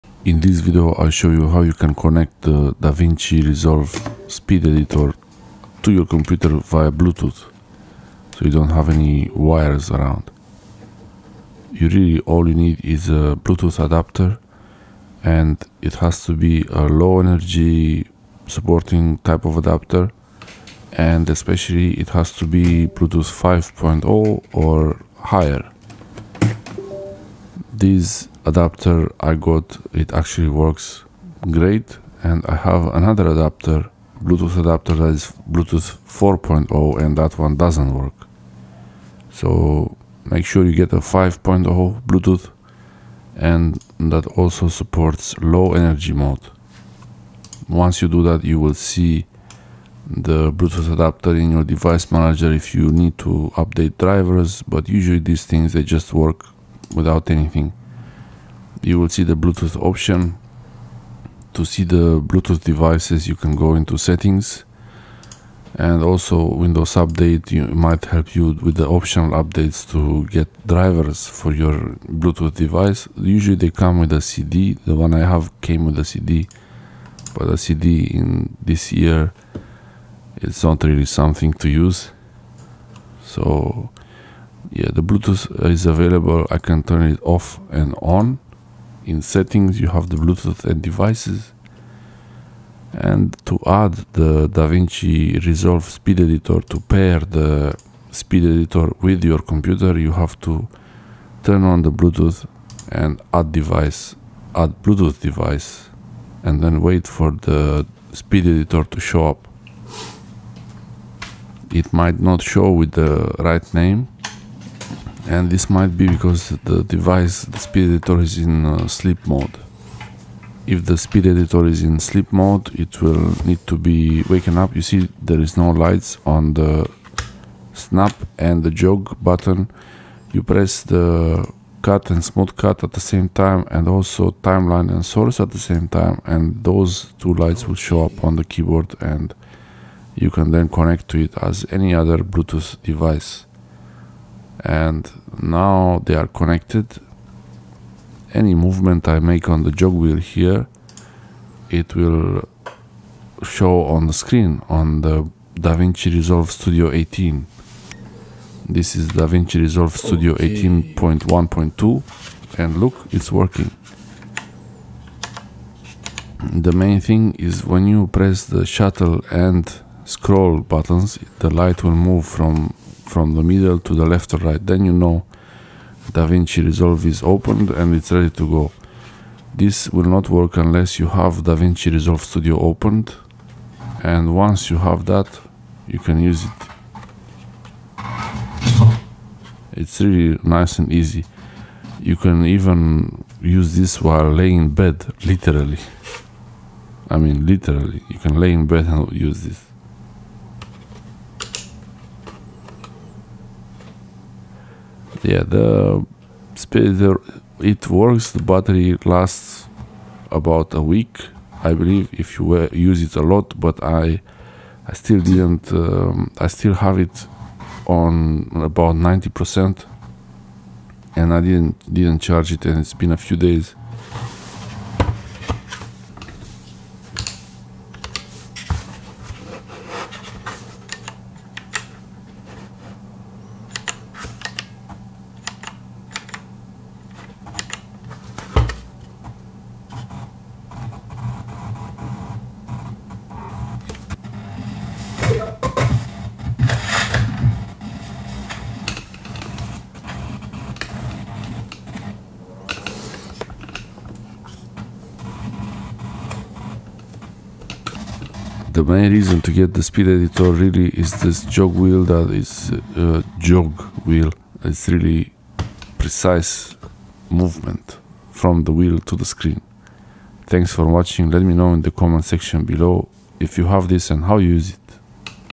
DaVinci Resolve Speed Editor Connect to Bluetooth Adapter PC
DaVinci Resolve Speed Editor keyboard connect to PC bluetooth adapter dongle.
What Bluetooth adapter dongle for PC works with DaVinci Resolve Speed Editor Keyboard.
How to connect to bluetooth on PC and how to wake-up DaVinci Resolve Speed Editor without USB cable.

The jog wheel of the speed editor feels really locked-in with the timeline or source monitor.
Any small slight movement on the jog wheel reflects in real time on the screen timeline as if there was a physical belt connecting the speed editor jog wheel and DaVinci Resolve Studio.

The rotating dial feels solid and weighty but very well balanced as seen in the video.
Jog wheel can function as a shuttle wheel which I personally don't like much and for fast scrolling thru the timeline.
The scroll feels really effective at going thru a lot of footage fast finding what is needed.
In the video you can see my first edit with the speed editor and how easy it is to use to put a timeline together.

The DaVinci Resolve Speed Editor comes with a full DaVinci Resolve Studio 18 license 18.1 at the moment
Not only that but you can activate one license on 2 computers at the same time.

Before first use, plug in the USB cable to charge 2-3 hours minimum.
Update the speed editor firmware when asked but only if the speed editor battery is more than 30%-50%.

I love it really love the solid feel of the wheel with rubbery edges.
Used it on 3-4 videos already and feels like learning to assemble a timeline with the speed editor is much easier than I initially thought.
Wish I'd purchased the speed editor earlier.
The speed editor has many functions for quick transitions and cut options.

Nobody this month. You can also become a member on this channel.

Any questions?